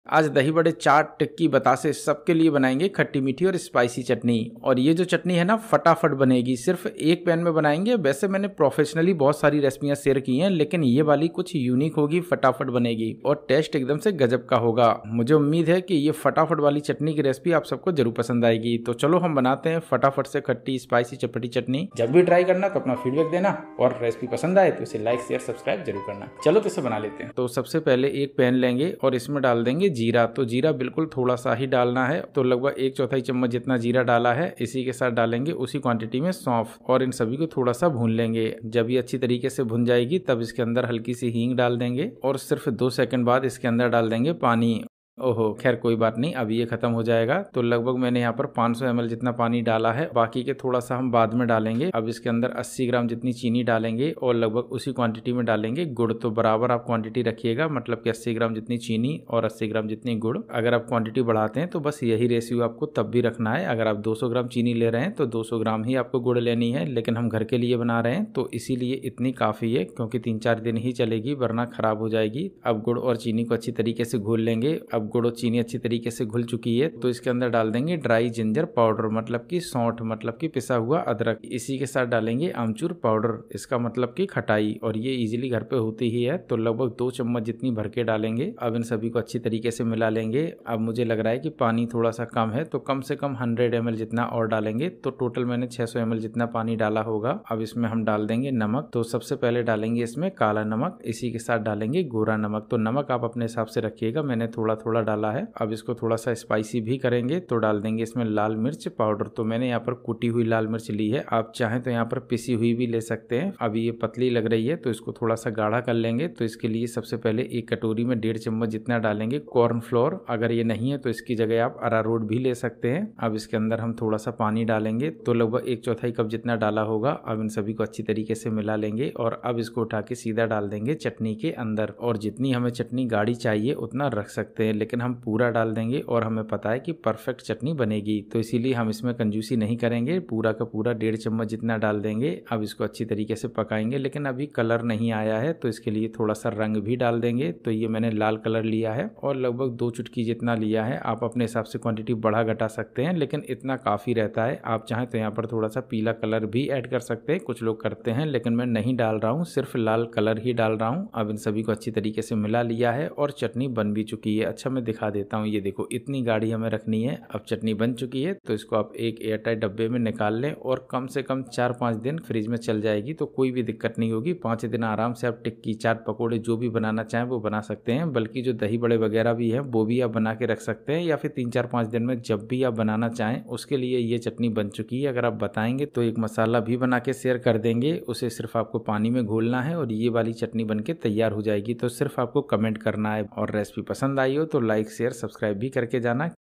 5 मिनट में बनाए खट्टी मीठी चटपटी चाट टिक्की समोसा वाली हलवाई स्टाइल चटनी - khatti meethi chatni
samosa wali chutney chat wali khatti meethi chutney chutney recipe meethi chutney sonth ki chatni